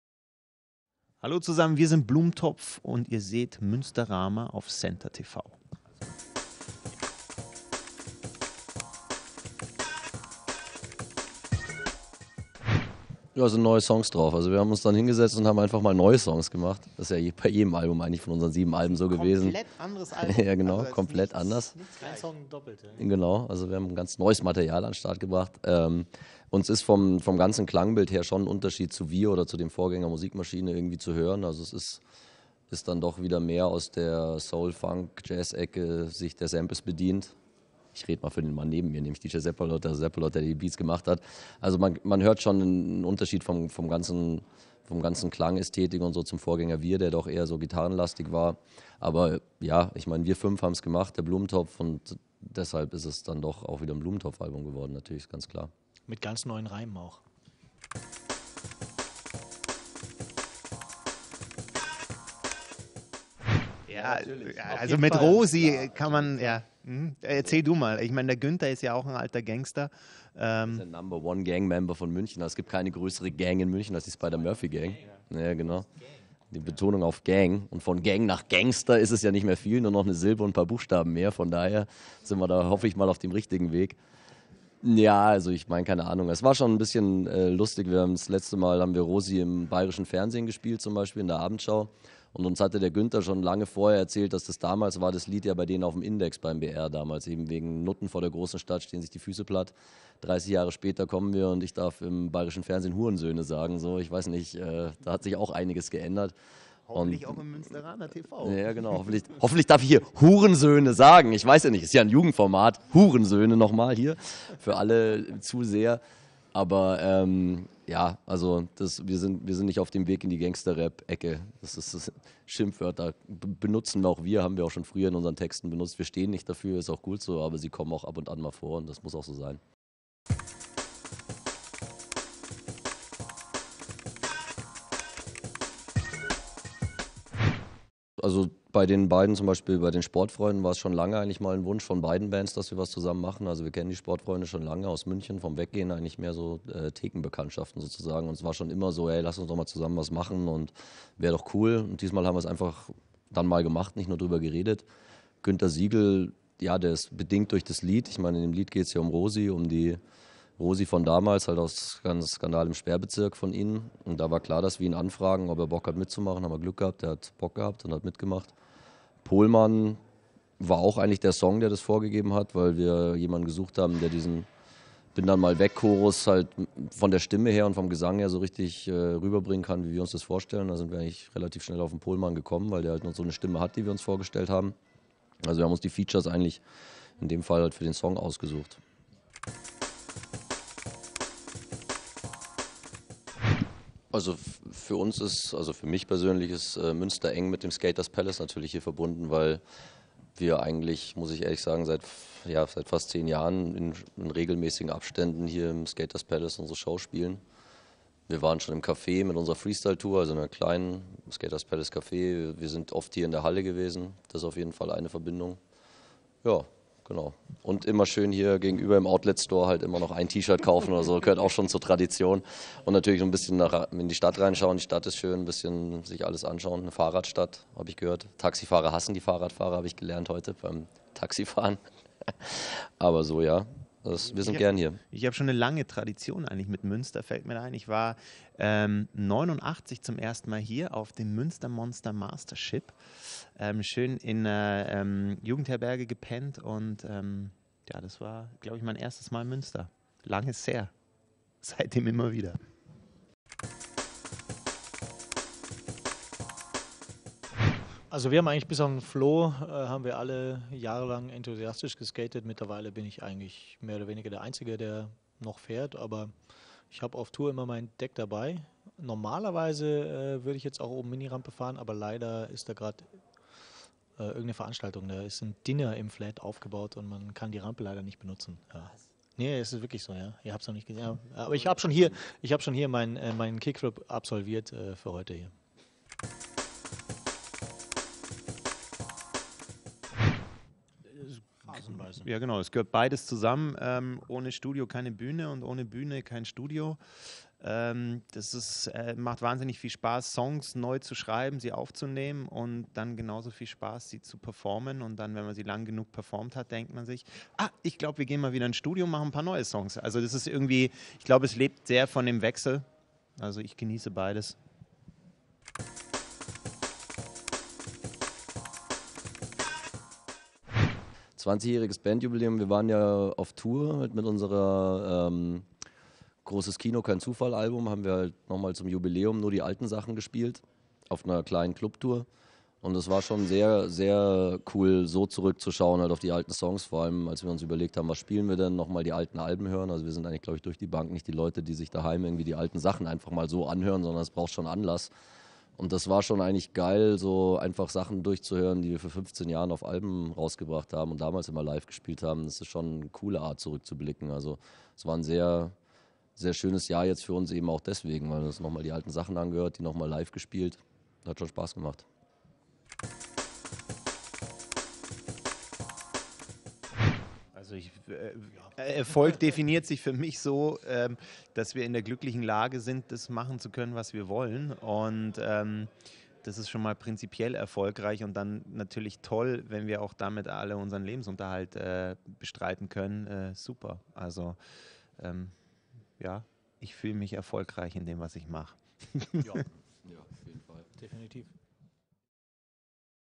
Münsterama - Das komplette Blumentopf Interview
Hier noch einmal das komplette Interview von Blumentopf vor deren Auftritt im Skater's Palace in Münster am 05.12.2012